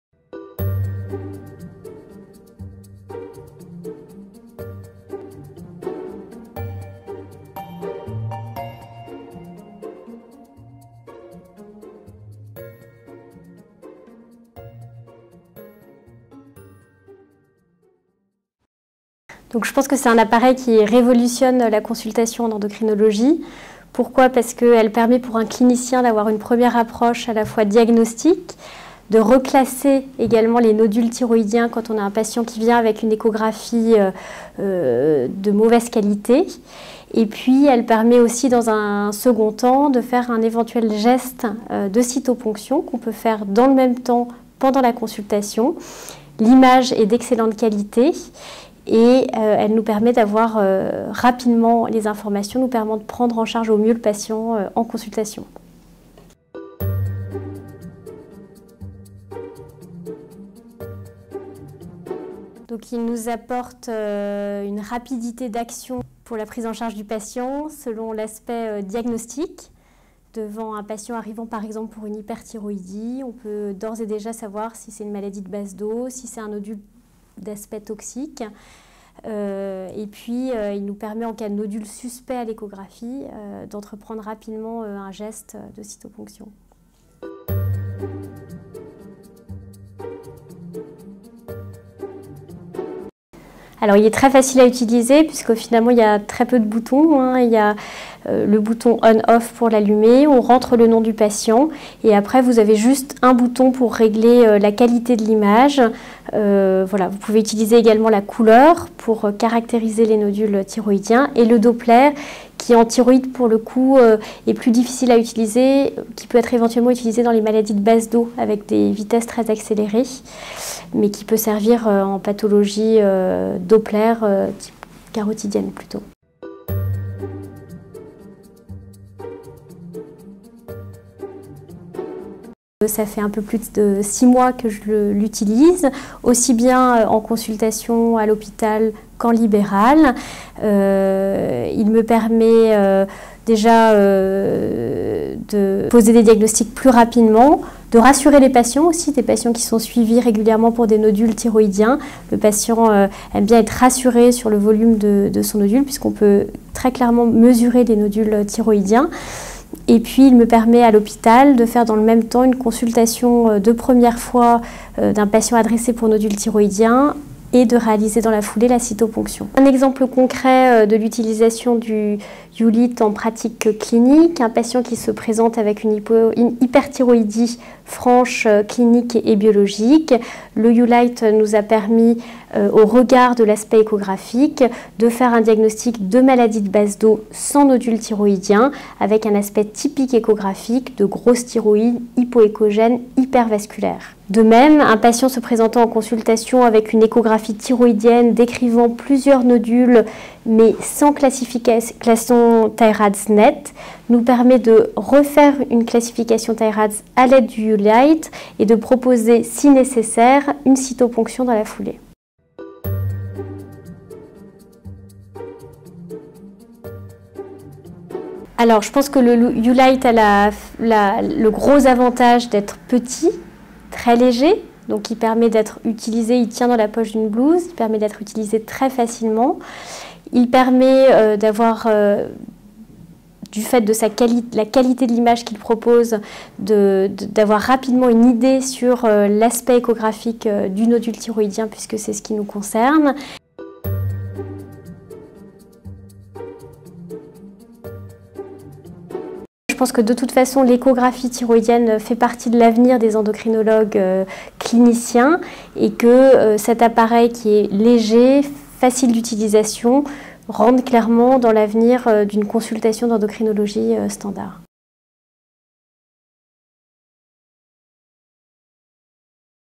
Dr Bricaire à propos du U-Lite en endocrinologie.mp4
Interview du Dr Bricaire à propos du U-Lite de Sonoscanner utilisé en Endocrinologie